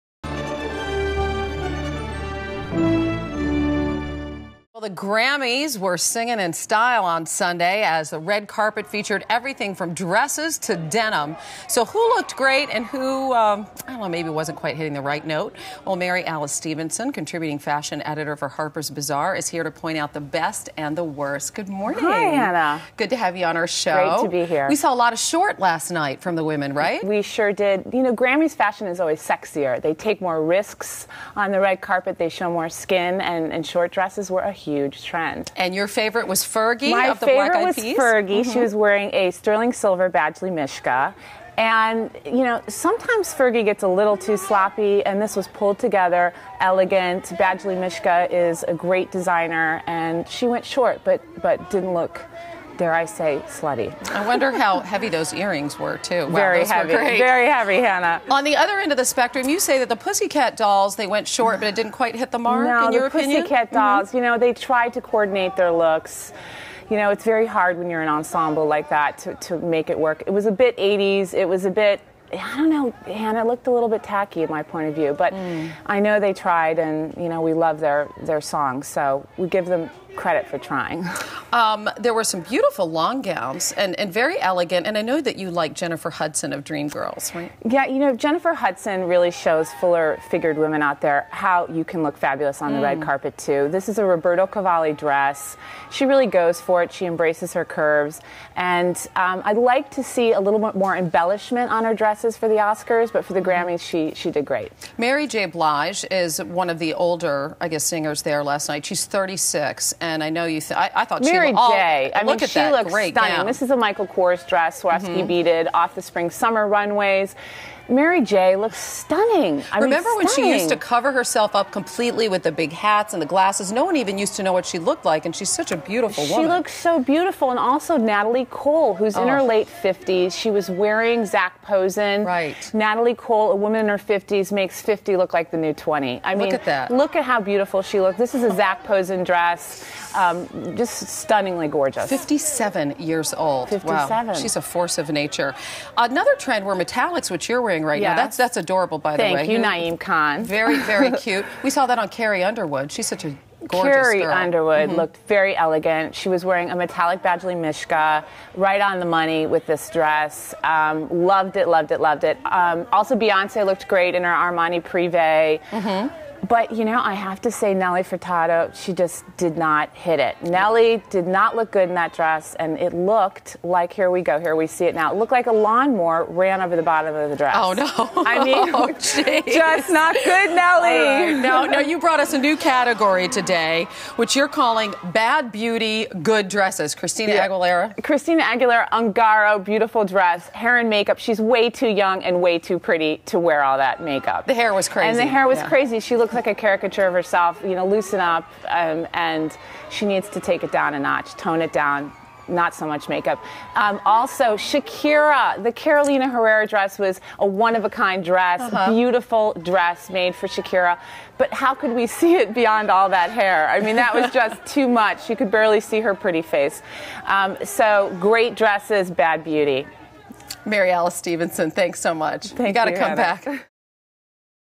Grammy Fashion Do's And Don'ts (CBS News)
The red carpet at the 49th annual Grammy Awards featured everything from dresses to denim. Mary Alice Stephenson, of Harper's Bazaar, recaps the night's hits and misses with Hannah Storm. (CBSNews.com)